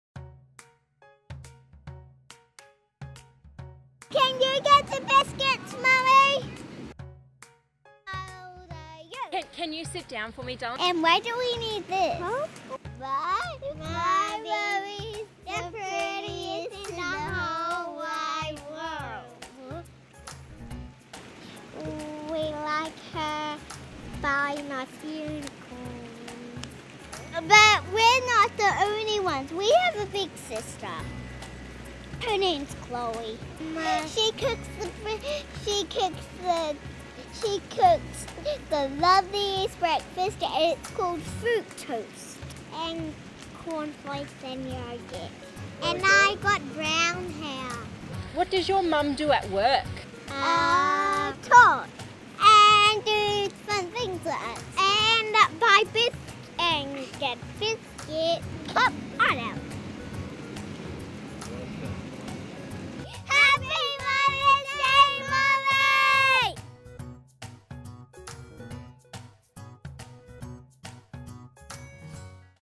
Mother's Day - Ella and Lucy
Ella and Lucy's mum Marlene works here in the Infrastructure Services Group.

Watch on for their hilarious reasons for why they love their mum. Happy Mother's Day to our mumsofthetoowoombaregion!